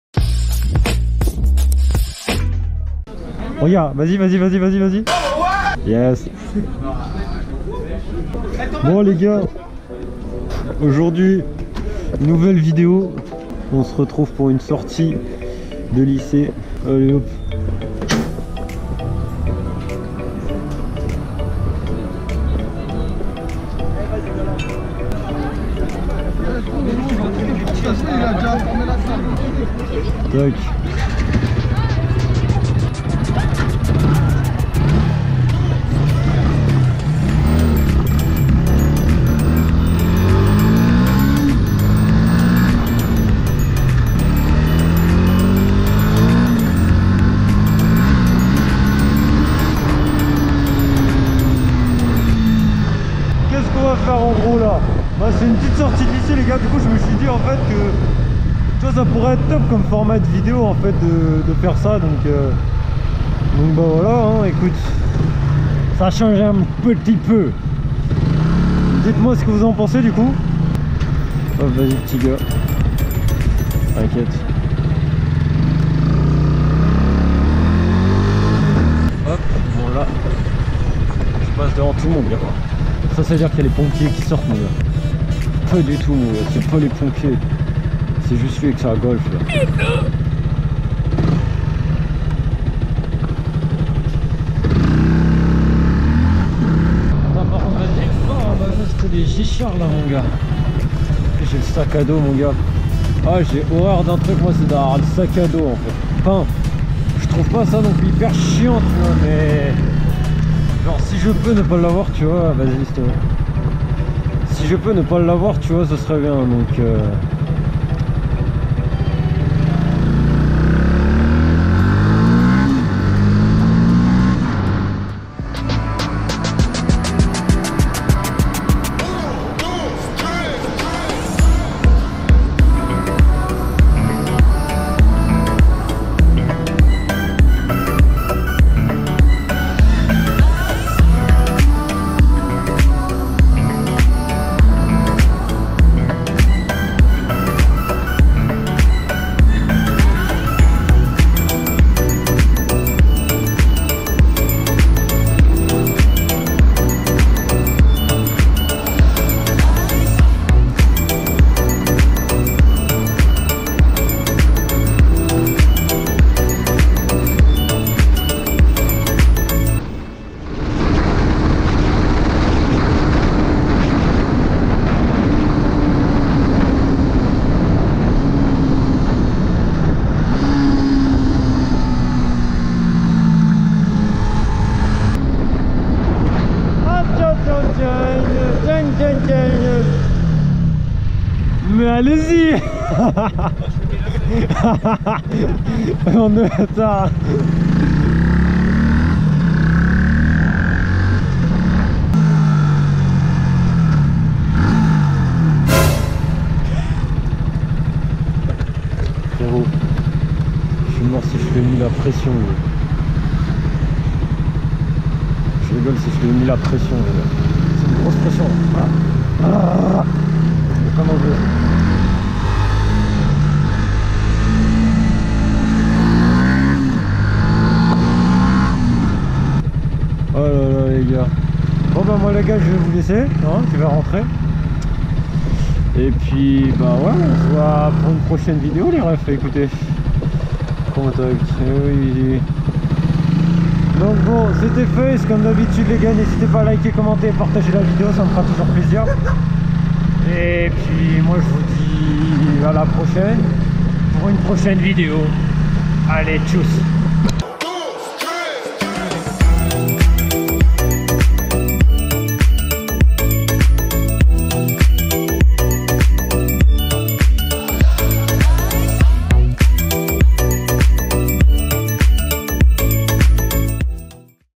dailyobservation en Yzf R125 #26 SORTIE DE LYCÉE EN R125 !!! 😁
Aujourd'hui nouvelle vidéo, journée sortie de lycée. J'espère que la vidéo vous plaira comme d'habitude n'hésite pas à liker, commenter et partager la vidéo ça fait toujours plaisir.

Ma moto 🏍 : Yzf R125 2018

Mon matos 🎬 :
Caméra : Go Pro hero 5 black
Micro : J'en ai pas 😂
Logiciel de montage : Adobe Première Pro  CC 2019